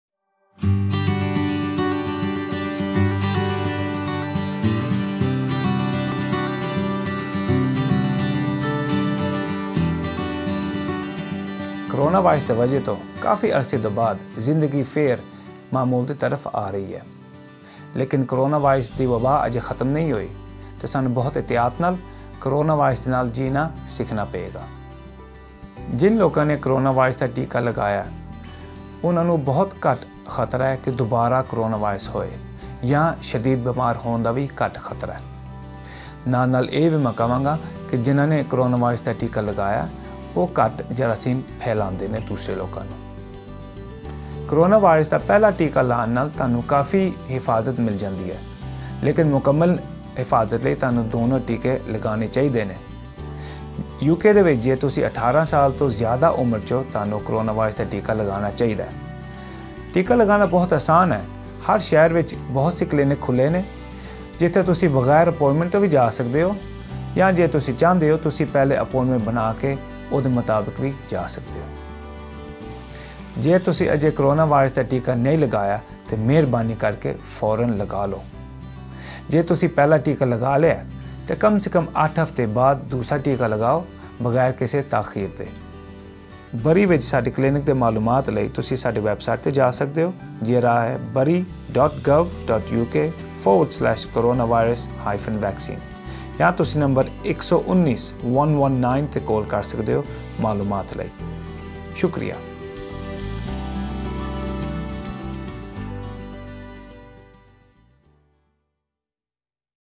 Punjabi version - Getting vaccinated against COVID-19
Punjabi version: If you haven’t yet had your first Covid-19 vaccine and are due, get booked in or access one of our walk-in clinics as soon as you can. Protect yourself and those around you